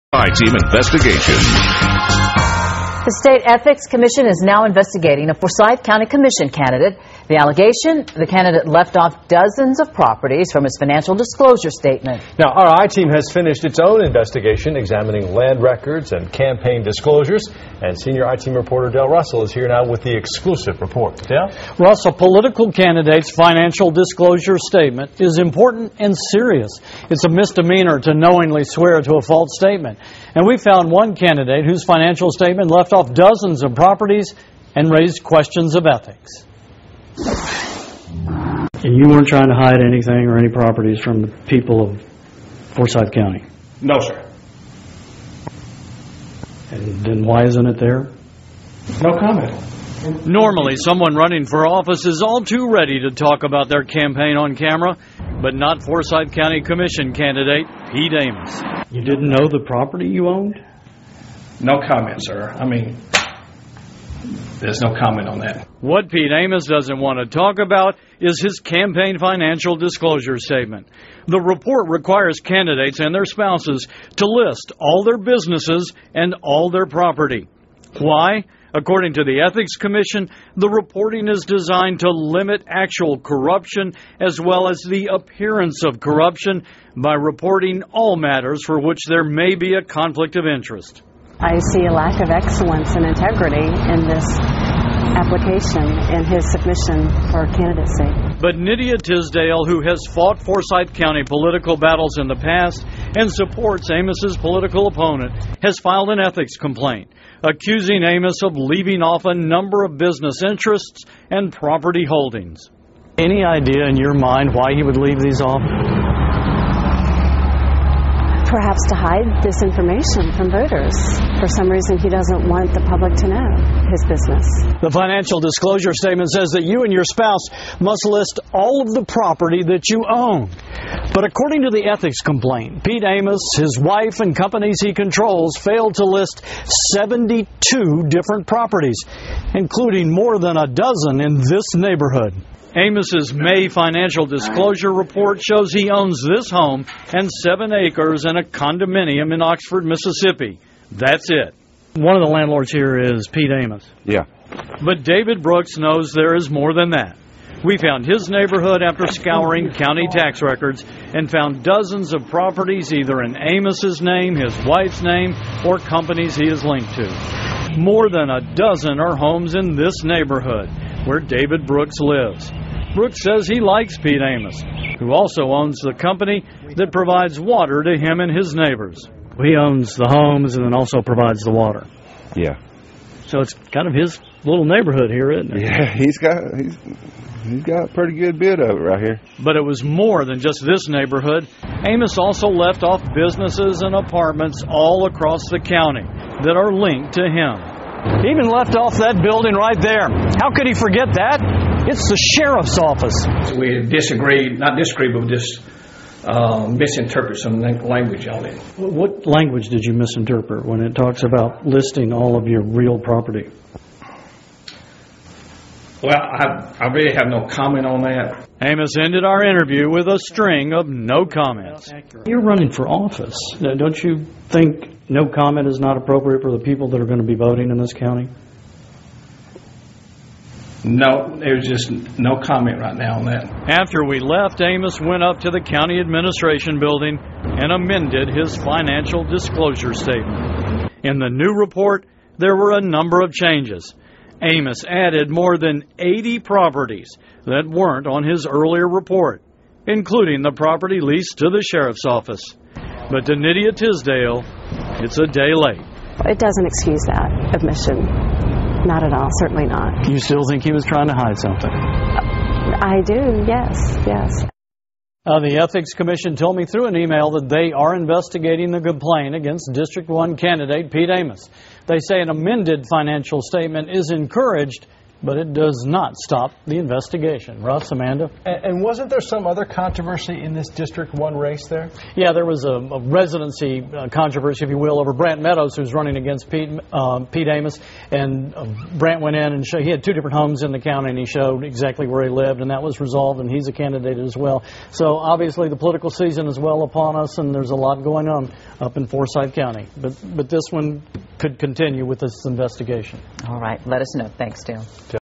Ethics complaint against Forsyth County Commission Candidate R.J. (Pete) Amos 06/24/10
Forsyth County Commission candidate Ralph J. (Pete) Amos speaks to Fox 5 I-Team investigative reporter Dale Russell about his campaign financial disclosure report in which he failed to disclose scores of properties on June 24, 2010.

Following a state ethics complaint filed by Nydia Tisdale, Amos submitted an amended financial disclosure statement.